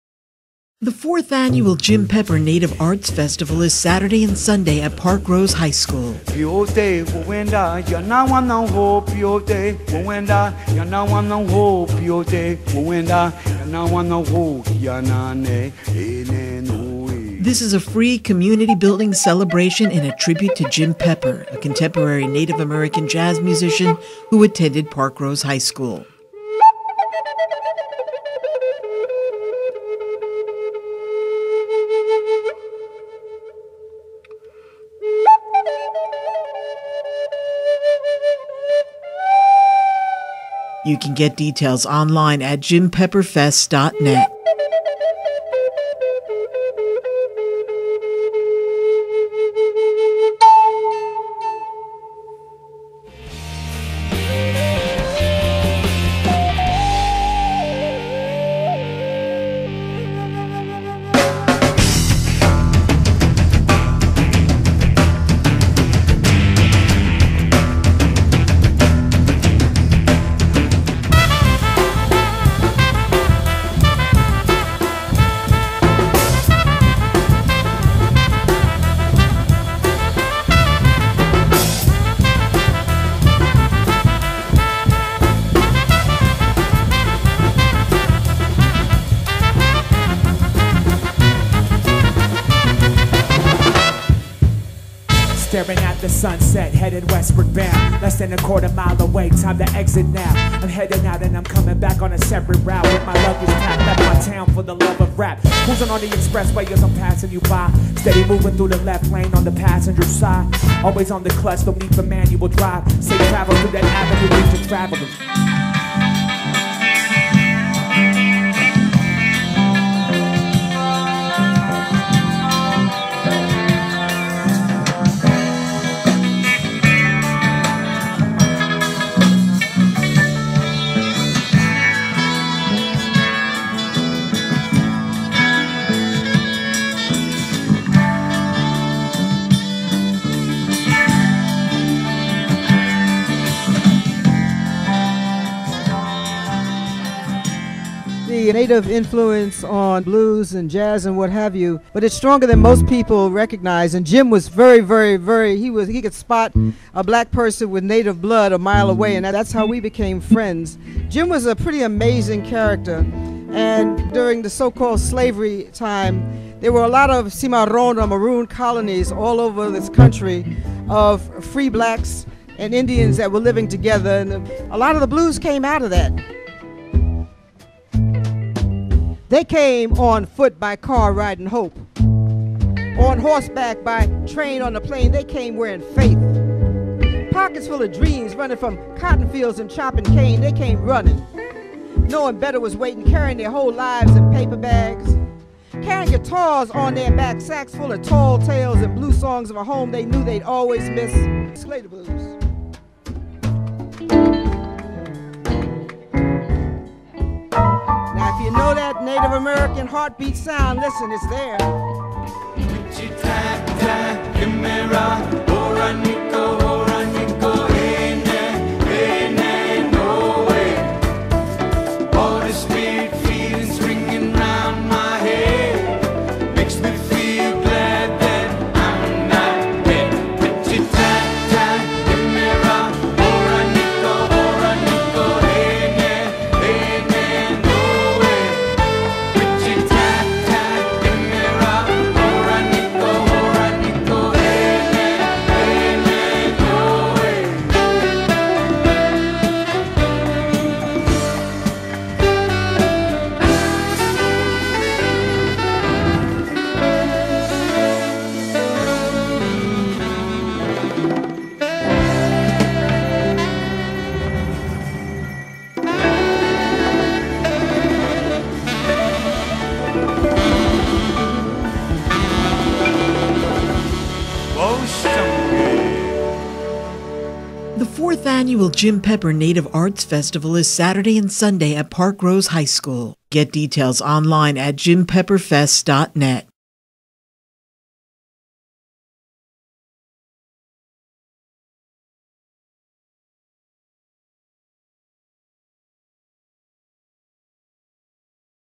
2016 Jim Pepper Fest Video
The 4th annual Jim Pepper Native Arts Festival is Saturday and Sunday September 24th and 25th, 2016 at Parkrose High School.
Everyone is welcome - admission is free